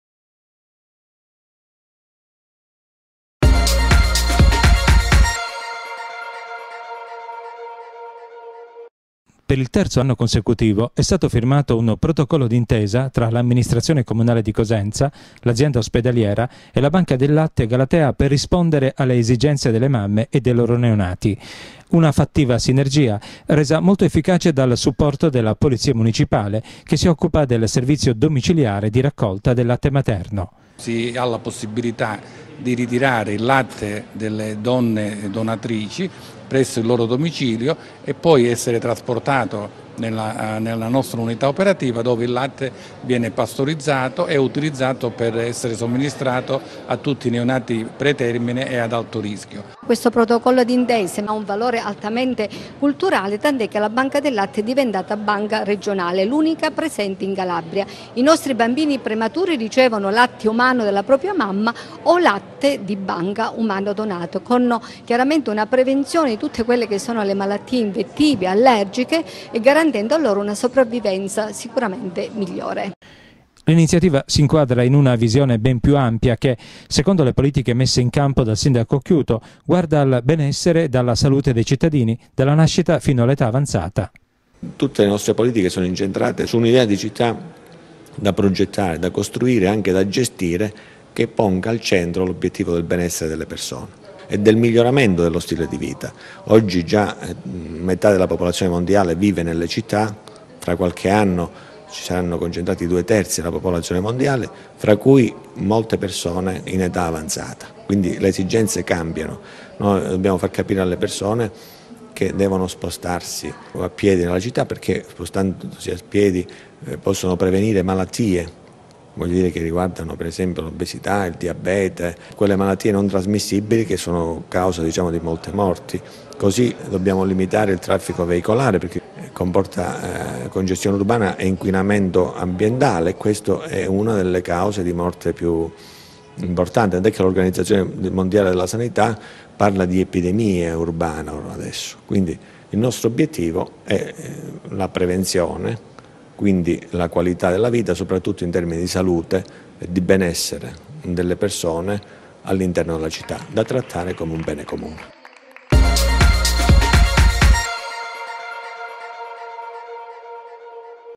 Cosenza, comune: rinnovato protocollo con la Banca del Latte
Il Comune di Cosenza, per il terzo anno, ha rinnovato il protocollo d'intesa con la banca del latte Galatea.